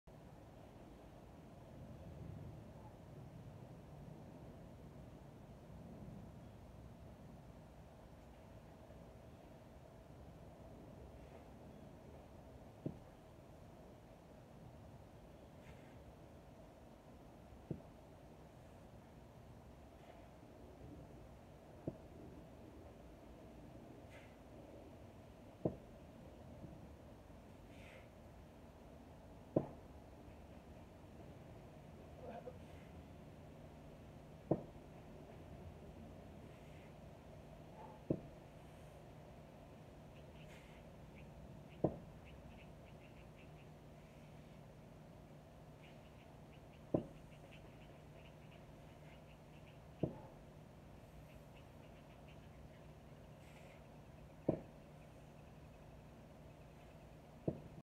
Kettlebell Gorilla Row
Keep core fully engaged with TVA drawn in, keep glutes and hamstrings firing to remain in a proper hinge, keep chest proud to prevent rounding the back, and draw head back in line with shoulders rather than allowing it to duck forward.  If you need to take a slight break to adjust the position of the hinge, go for it.  Finally, drive the non-rowing hand down into the ground and keep your hips squared to the ground as you perform each row without rotating.